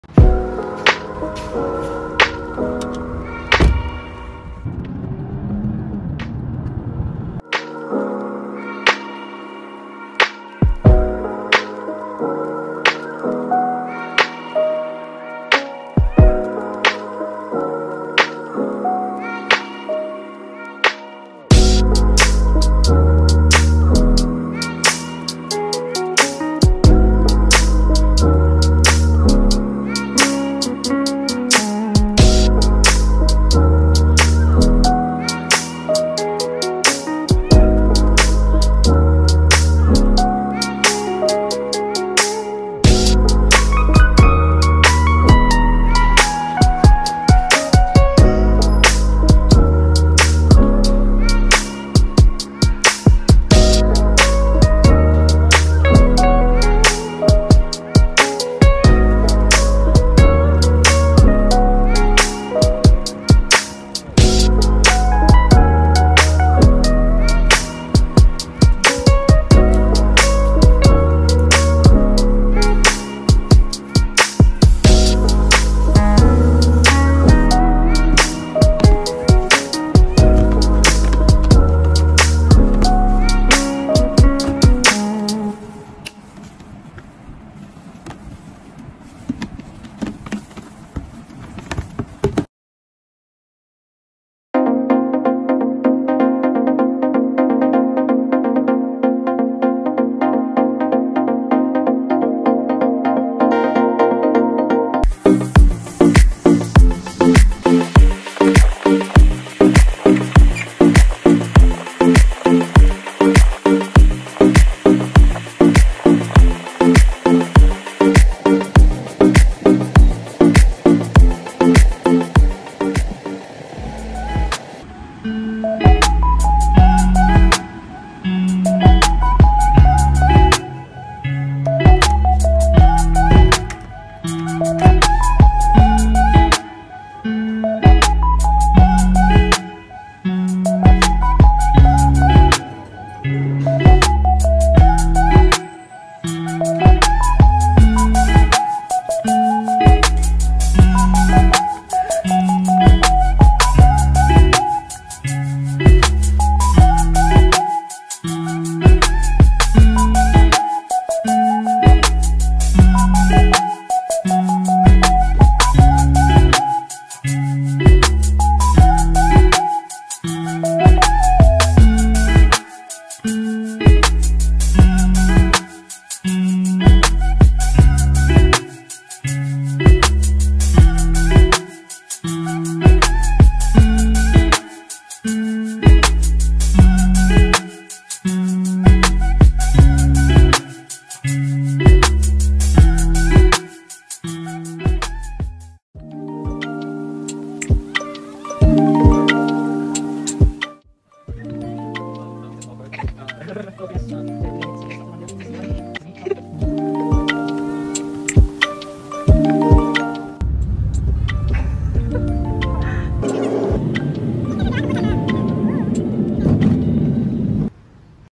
Registration Day at University of Malaya of a Medical Student
Hi guys, welcome to my first vlog ever!
I am a 2nd year medical student in University of Malaya. We are finally going back to campus after 1 year of online learning due to the COVID-19 pandemic.
I hope you like this vlog. :D

Also do check out the other videos on my channel. As always, stay safe and take care. ;)